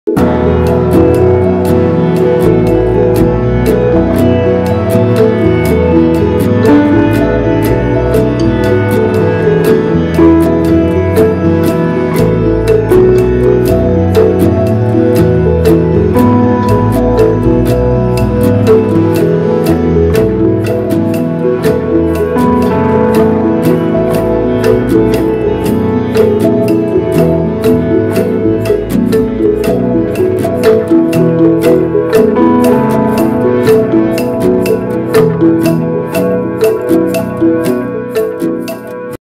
[ NAIL'TV ] 살롱와치 압구정의 힐링플레이스! 네일유메
유명 살롱을 탐방하는 코너인 살롱와치.
압구정의 힐링 플레이스 네일유메를 둘러 보았습니다.
늘 셀럽들의 사랑을 받는 네일 유메답게 아늑하고 아름다운 샵 분위기와 더불어 눈이 휘둥그레해지는 멋진 아트들이 가득한 샵이었습니다.